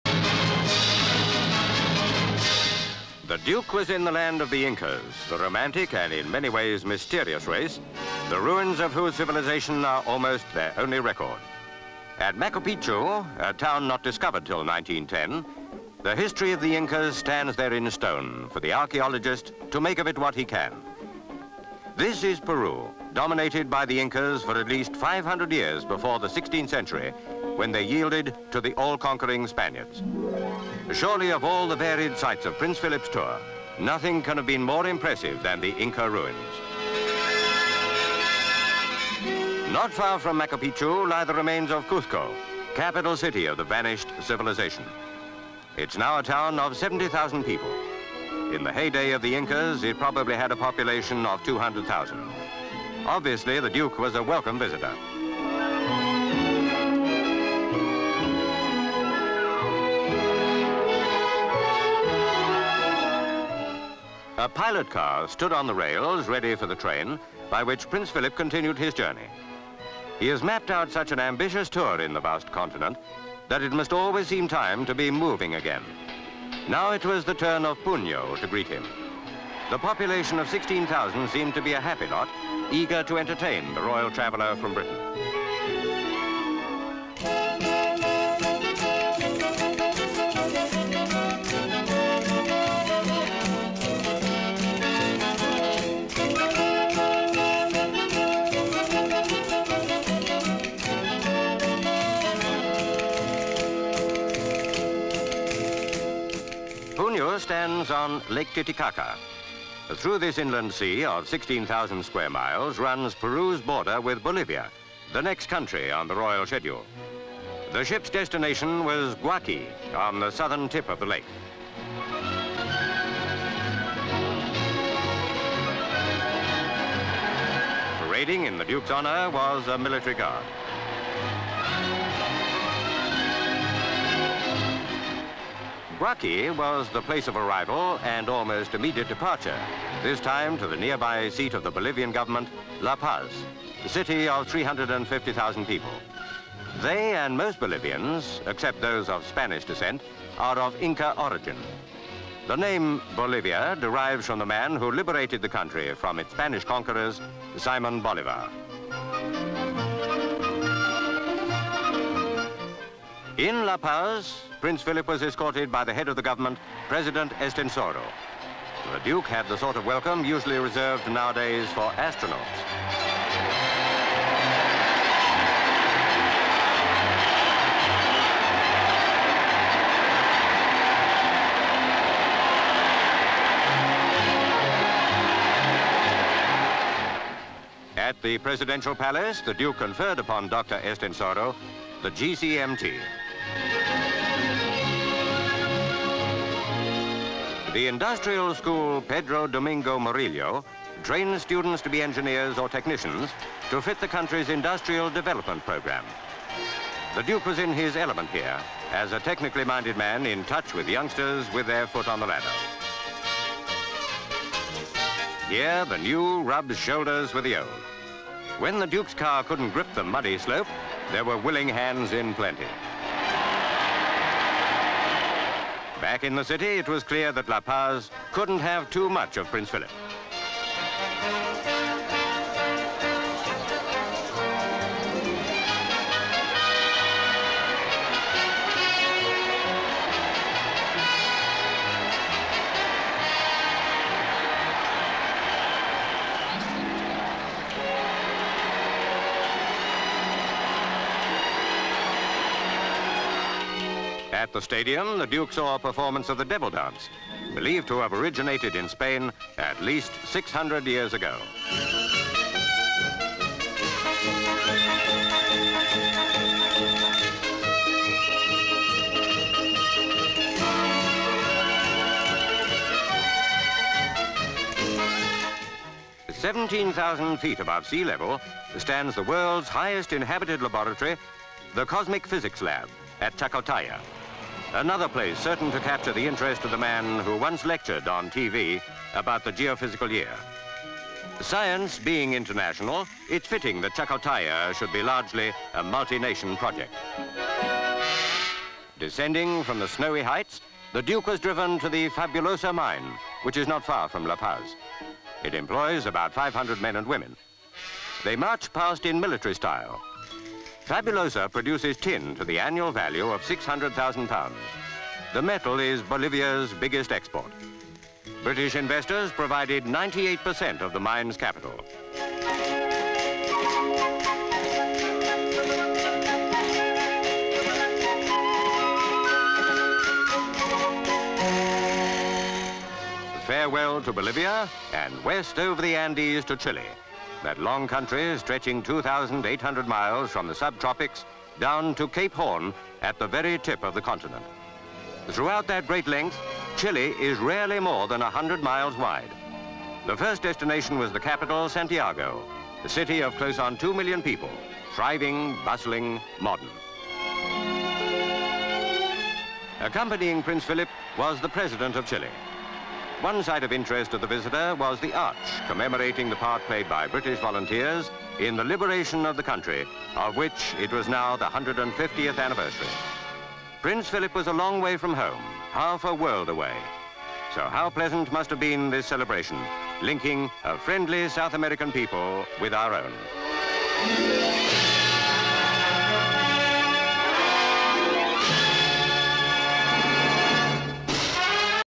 Duke's Successful Tour Aka Duke's Tour Of South America (1962)
Title reads: "Duke's Successful Tour".

Duke's Tour of South America - Peru, Bolivia and Chile.

M/S Inca boy presenting bouquet and hat to Duke of Edinburgh (Prince Philip).  Various shots of ruined Inca city of Machupicchu, Peru, mountain in distance.  We see Duke with party of officials touring around ruins.  M/S Inca couple sitting on ruins.  More shots Duke and party climbing to highest point of ruins.  C/U small Inca child.  M/S Duke and Party standing on edge of mountain overlooking city of Cuzco, Peru.  L/S large church in Cuzco, crowds are assembled in square to greet to Duke.  Various shots traditionally dressed Inca people in crowd.  C/U Duke shaking hands with Catholic priests.

Various shots royal train with pilot car on rails, it is waiting at Juliaca to take the Duke to Puno, Peru.  We see the Duke board the train.  Various travel shots from moving train.  M/S crowds in streets of Puno waving flags.  Duke and party walk down main street.  Various shots Duke and President of Peru watching traditional dancing in main square.  One of the dances looks like a Maypole dance.

L/S of Lake Titicaca.  M/S of river boat "Ollanta".  C/U Duke of bridge with captain.  The boat crosses the lake, crossing border from Peru to Bolivia.  Various shots Duke arriving in Guaqui, crowds and guard of honour wait on quayside.  M/S Duke waving from door of train taking him to La Paz.  Various street scenes in La Paz with Inca people selling fruit and vegetables from stalls.  Various shots of ticker tape parade through city.  Duke and President Estenssoro of Bolivia ride in convertible car.   L/S presidential palace.  Various shots Duke presenting GCMG to Dr. Estenssoro.

L/S industrial school of Pedro Domingo Murillo.  Various shots Duke touring school. Duke talks to some of the students. Boys help push Duke's car up muddy slope.   Various shots Duke walking through La Paz, women through confetti over him.    Various shots Duke watching traditional masked "Devil Dance" in sports stadium.  Various shots Duke visiting Cosmic Physics Lab on mountain top, it snows heavily.  Various shots of Duke visiting Fabulosa tin mine.  Miners march past Duke in military style.

Aerial shots of Andes mountains. Various shots Santiago, Chile.  Various shots Duke and President walking through towns.  Various shots Duke unveiling plaque on arch commemorating liberation of Chile.

Please note: the selected originals (cuts, rushes, out-takes) for this story are in colour.
 FILM ID:1713.19